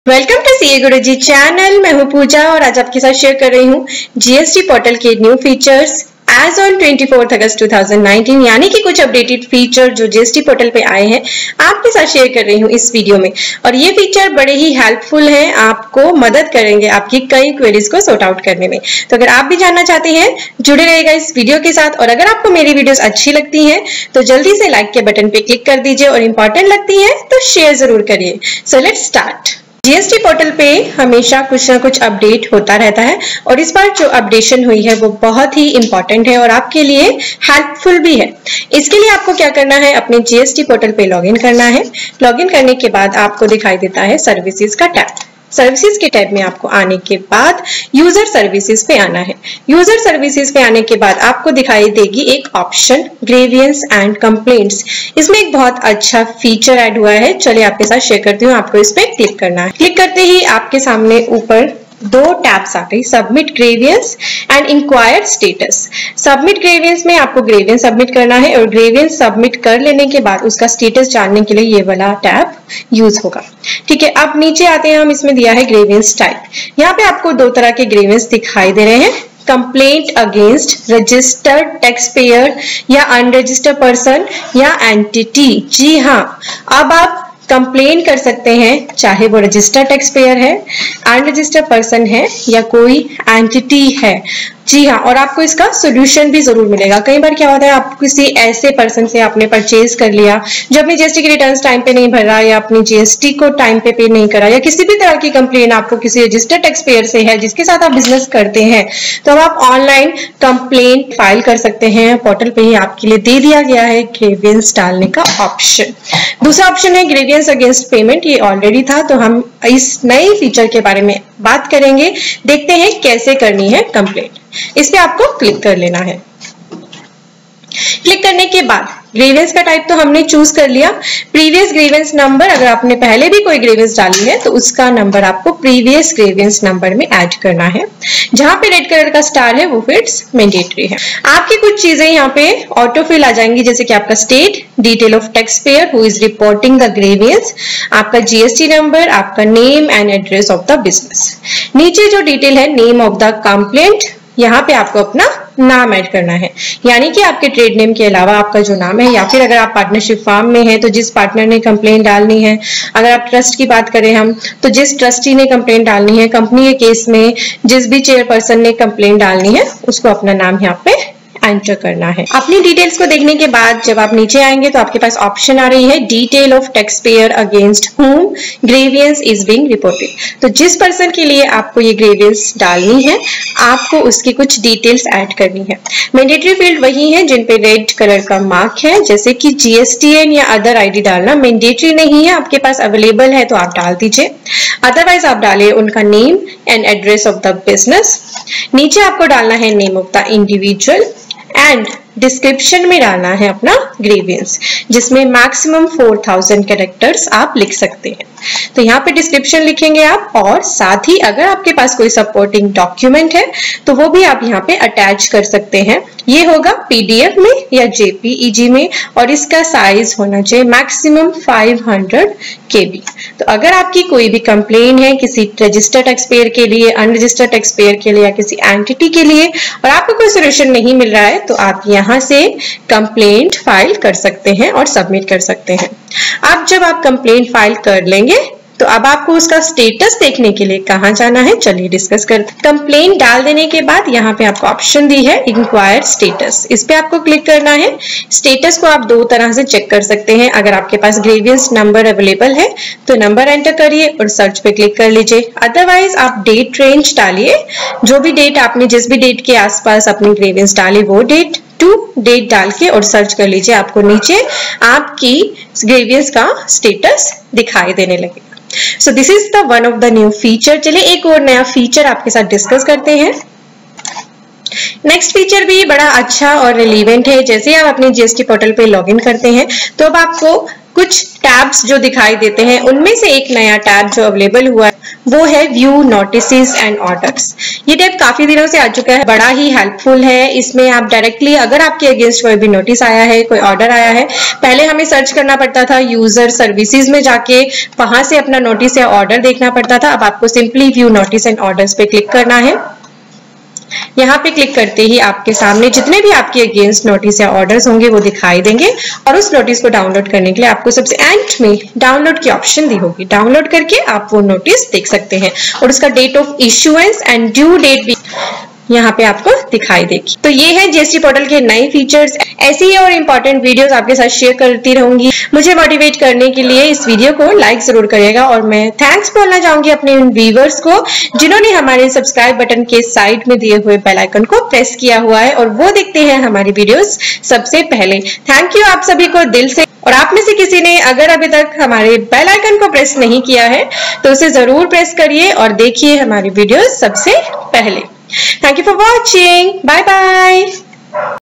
New Features on GST portal as on 24th AUGUST 2019 (YOU WILL LOVE THESE)
GST portal is launched new feature of complaint against registered taxpayers/unregistered/entity which can help you if you are facing any issue. watch video to know more

(only for paid services)

♥Mike for Audio recording

♥Camera Stand

♥♥♥WATCH OUR MOST LOVED & IMPORTANT VIDEOS♥♥♥